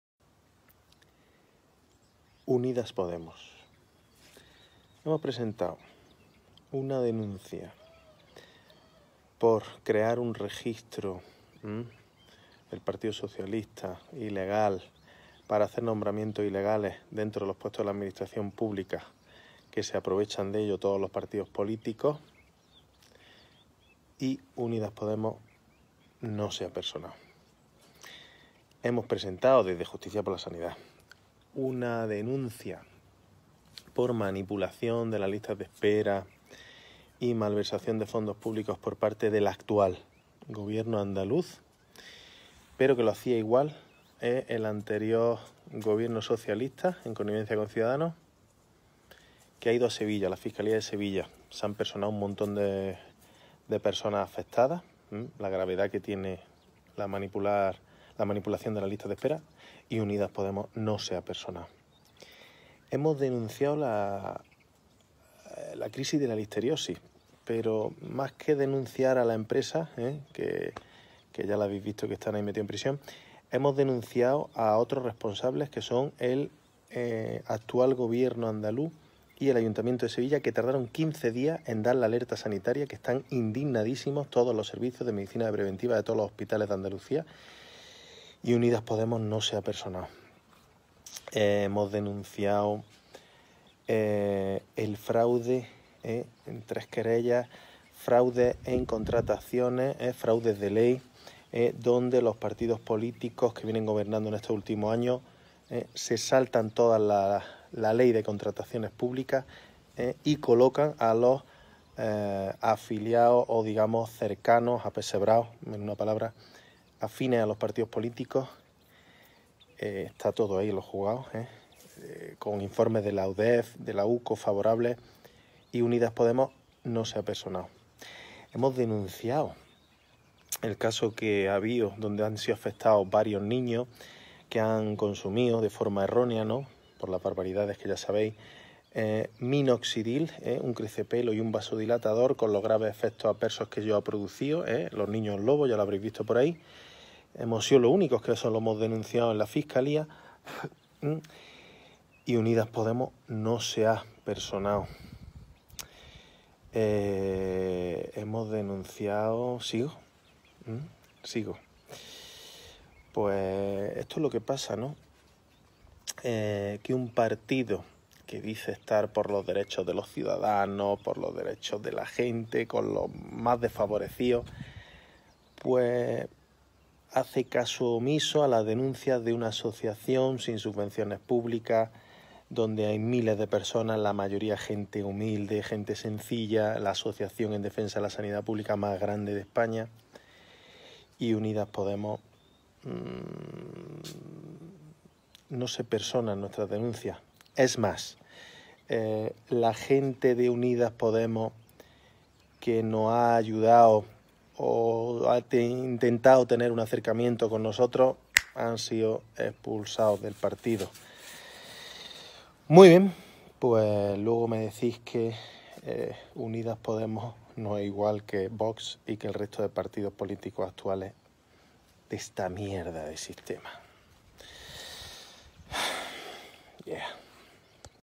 Unidas Podemos no se ha personado.
Mensaje para Unidas Podemos, otro fraude más de esta partidocracia tóxica para los que no somos apesebraos ni analfabetos funcionales. 😎yeah